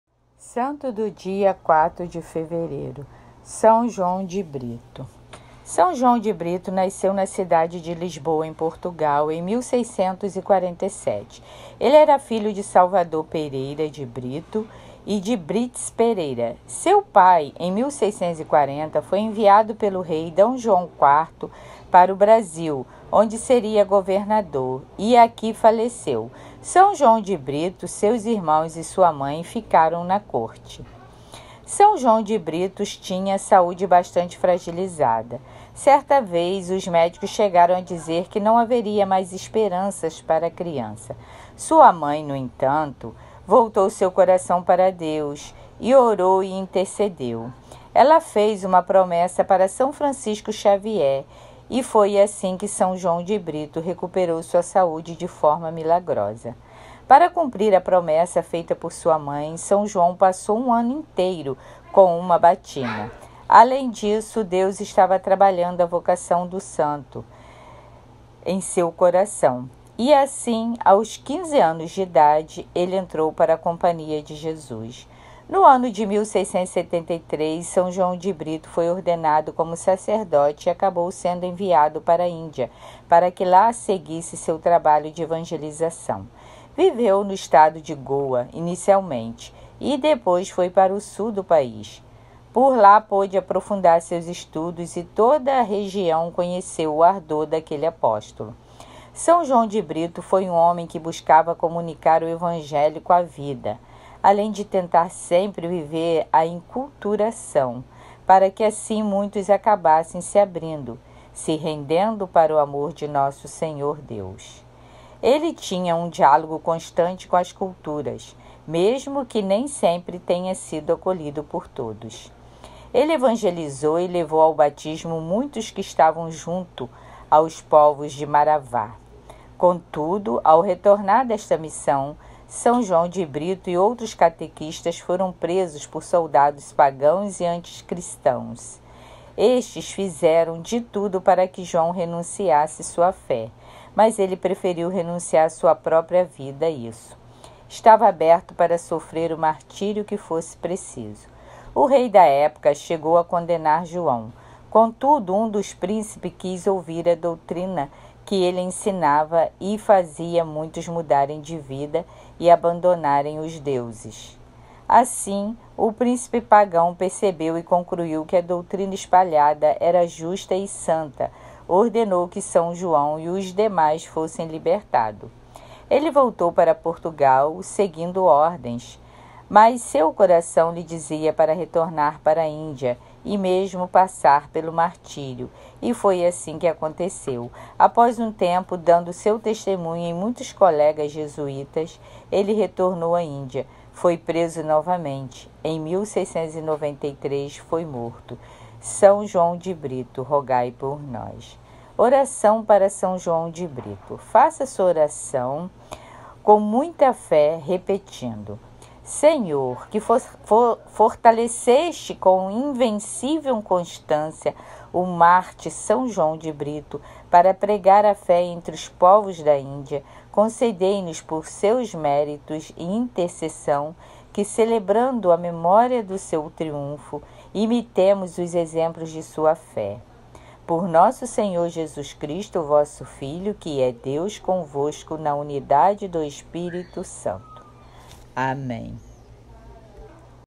HISTÓRIA E ORAÇÃO DE SÃO JOÃO BRITO
O Santo do Dia 04 de fevereiro nasceu na cidade de Lisboa, em Portugal, em 1647. Ele era filho de Salvador Pereira de Brito e D. Brites Pereira. Seu pai, em 1640 foi enviado pelo rei Dom João IV para o Brasil, onde seria governador, e aqui faleceu. São João de Brito, seus irmãos e sua mãe ficaram na corte.
São João de Brito tinha a saúde bastante fragilizada. Certa vez os médicos chegaram a dizer que não haveria mais esperanças para a criança. Sua mãe, no entanto, voltou seu coração para Deus e orou e intercedeu. Ela fez uma promessa para São Francisco Xavier e foi assim que João de Brito recuperou sua saúde de forma milagrosa. Para cumprir a promessa feita por sua mãe, São João passou um ano inteiro com uma batina. Além disso, Deus estava trabalhando a vocação do santo do dia 04 de fevereiro em seu coração e assim, aos 15 anos de idade, ele entrou para a Companhia de Jesus.

No ano de 1673 São João de Brito foi ordenado como sacerdote e acabou sendo enviado para a Índia, para que lá seguisse seu trabalho de evangelização. Viveu no estado de Goa inicialmente e depois foi para o sul do país. Por lá pode aprofundar seus estudos e toda a região conheceu o ardor daquele apóstolo. São João de Brito foi um homem que buscava comunicar o Evangelho com a vida, além de tentar sempre viver a inculturação, para que assim muitos acabassem se abrindo, se rendendo para o amor de Nosso Senhor, Deus. Ele tinha um diálogo constante com as culturas, mesmo que nem sempre tenha sido acolhido por todos.

Ele evangelizou e levou ao batismo muitos que estavam junto aos povos de Maravá. Contudo, ao retornar desta missão, São João de Brito e outros catequistas foram presos por soldados pagãos e anticristãos. Estes fizeram de tudo para que João renunciasse sua fé, mas ele preferiu renunciar sua própria vida a isso. Estava aberto para sofrer o martírio que fosse preciso. O rei da época chegou a condenar João, contudo um dos príncipes quis ouvir a doutrinar que ele ensinava e que fazia muitos mudarem de vida e abondarem os deuses. Assim, o príncipe pagão percebeu e concluiu que a doutrina espalhada era justa e santa e ordenou que São João e os demais fossem libertados.

Ele voltou para Portugal, seguindo ordens, mas seu coração lhe dizia para retornar para a Índia e mesmo passar pelo martírio, e foi assim que aconteceu. Após um tempo, dando seu testemunho em muitos colegas jesuítas, ele retornou à Índia. Foi preso novamente. Em 1693 foi morto.

Oração para São João de Brito

Faça sua oração para o santo do dia de hoje, com muita fé, repetindo:

Senhor, que fortaleceste com invencível constância o mártir São João de Brito para pregar a fé entre os povos da Índia, concedei-nos, por seus méritos e intercessão, que, celebrando a memória do seu triunfo, imitemos os exemplos da sua fé. Por Nosso Senhor Jesus Cristo, Vosso Filho, que é Deus convosco na unidade do Espírito Santo. Amém.